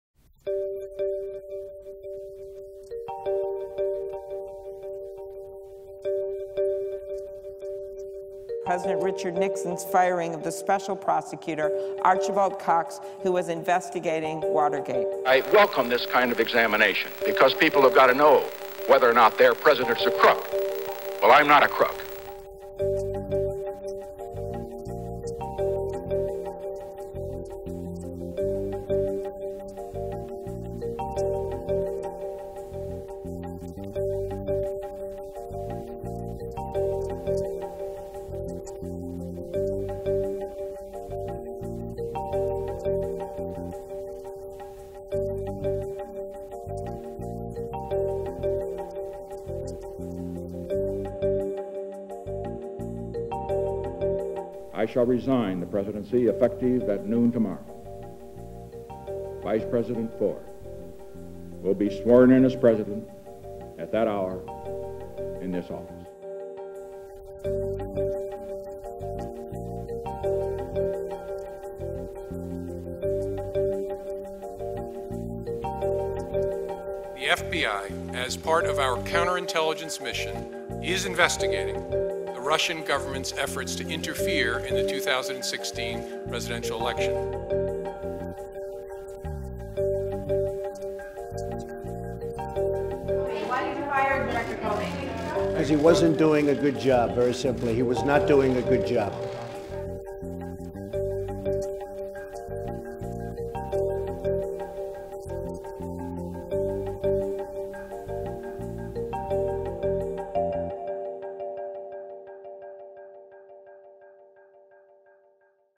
Why Everyone Is Comparing President Donald Trump's James Comey Firing To Richard Nixon | TIME
When President Trump fired FBI Director James Comey Tuesday, people were quick to cry Nixon.

Trump's dramatic decision does have historical echoes to Nixon's presidency: Trump fired Comey, who was overseeing an investigation into his campaign's possible ties to Russia. President Nixon also fired someone investigating him.

In 1973, in what would become known as the Saturday Night Massacre, Nixon ordered the firing of Archibald Cox, a special prosecutor looking into the burglary at the tip of the iceberg of the Watergate scandal. Both Nixon's attorney general and deputy attorney general resigned in protest, and the "massacre" marked a key step in the scandal that led to his resignation less than a year later.